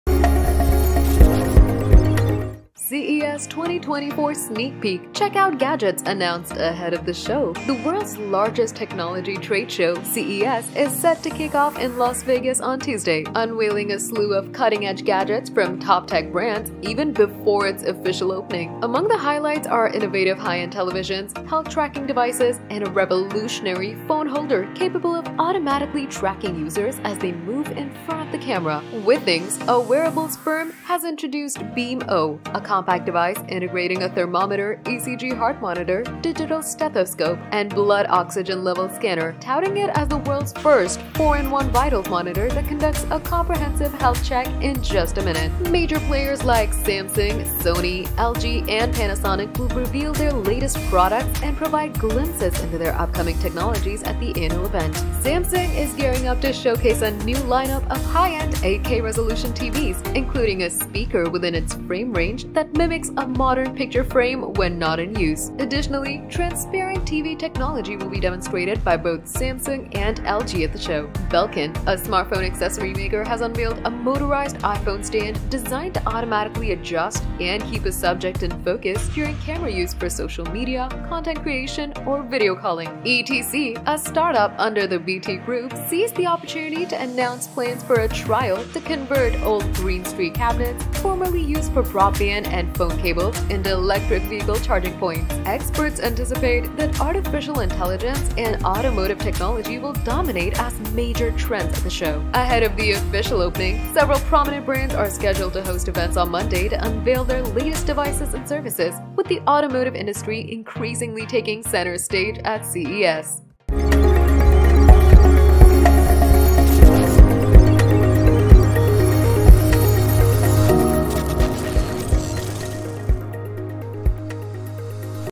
CES 2024: Everything revealed in 2-minutes
The world's largest technology trade show, CES, is set to kick start  in Las Vegas today, unveiling a slew of cutting-edge gadgets from top tech brands even before its official opening.

Among the highlights are innovative high-end televisions, health-tracking devices, and a revolutionary phone holder capable of automatically tracking users as they move in front of the camera.